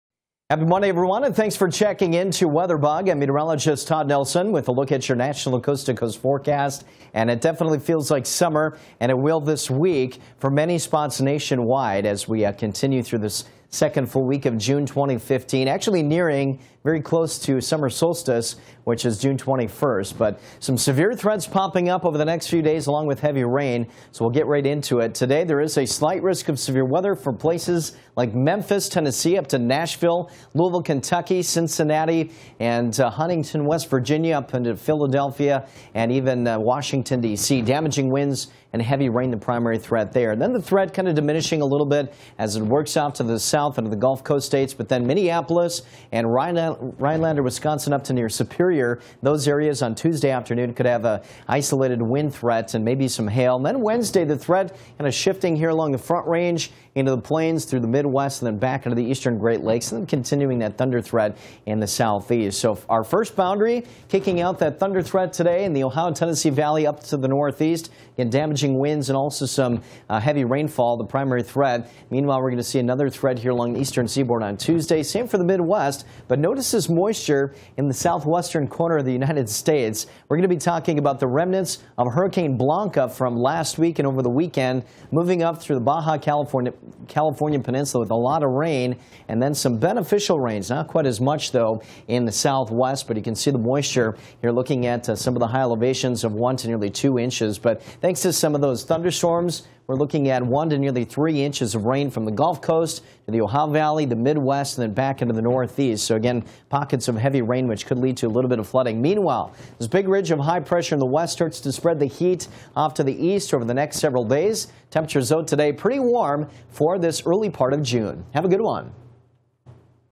Monday's Weather Outlook
Big storms will cause trouble across the Ohio Valley, central Appalachians and the Mid-Atlantic Today. The Southwest stays hot.